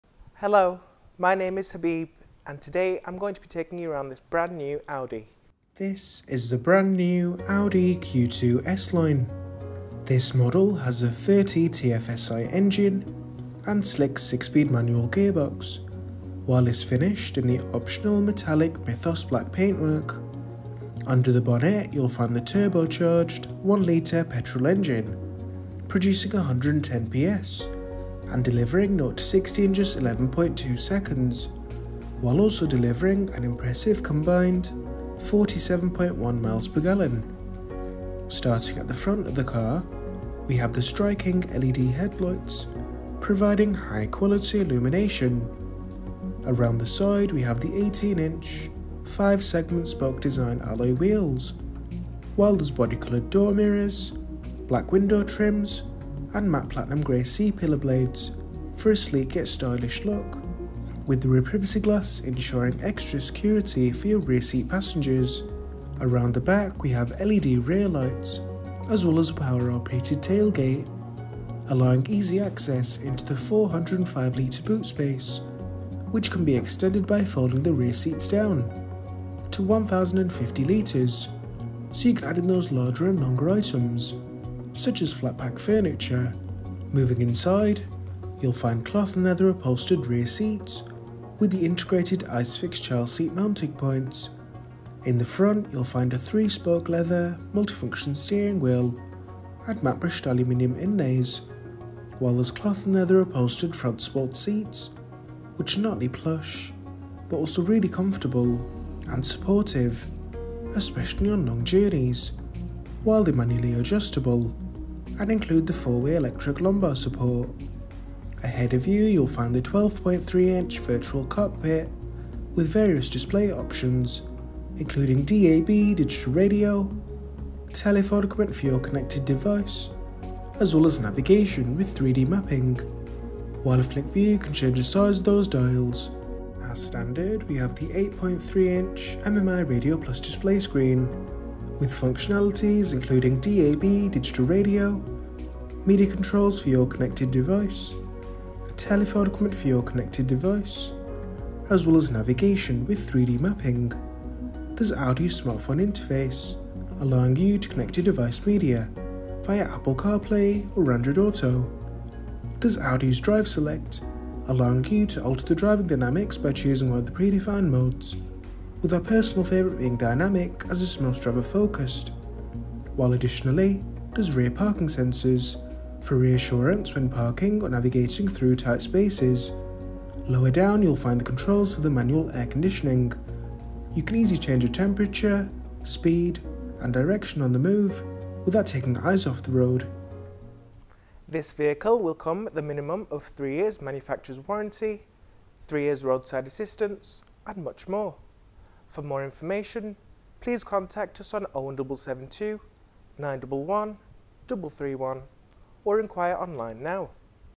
Brand New Audi Q2 1.0 TFSI 30 S line 5dr | Preston Audi
Just into our stock, this Brand New Audi Q2 1.0 TFSI 30 S line 5dr in Mythos Black, is now available for sale from Preston Audi.

Brand New
110ps 30 TFSI
Petrol
Manual

Main Features include:
12.3" Virtual Cockpit
8.3" MMI Navigation Plus Display Screen

0:00 - Intro
0:05- Exterior Tour
1:06- Boot Tour
1:27- Rear Seats Tour
1:35- Interior Tour
3:10- How To Get In Touch